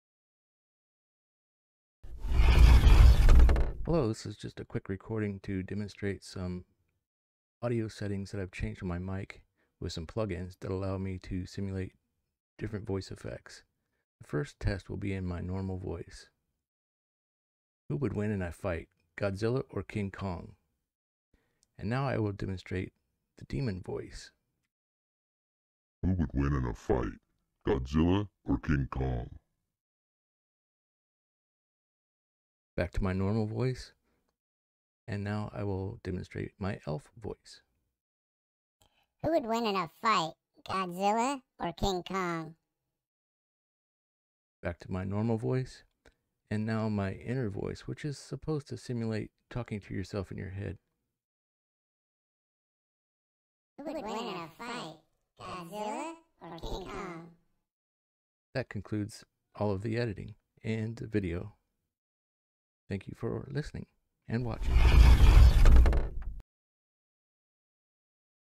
Free OBS Voice FX
Demonstration of some voice effects that I have applied to my OBS Studio microphone settings using free VST plugins.

Please join in to my live stream performances!

Please watch the Custom Voice Changers video linked below on the rStation YouTube channel as it explains where to download the necessary plugins and instructions for installing the plugins within your OBS Studio:

Below are the specific VST plugins and settings that were used to create the different voice FX used in my video:

Demon voice effect
VST Plugin: GForm
Pitch: -7.0
Formant: -6.0
Frequency: 50, 800, -70dB

Elf voice effect
VST Plugin: GForm
Pitch: 10.0
Formant: 10.0
Frequency: 60, 500, -70dB

Inner voice effect (require the VST plugin for the Elf voice and the plugin shown below in the mic filter settings)
VST Plugin: GSpace
Phase Widen: On
Gain: -9.0dB
Mid/Side: 0dB
Delay: 40ms
Feedback: -8
Dry: -10
Wet: 0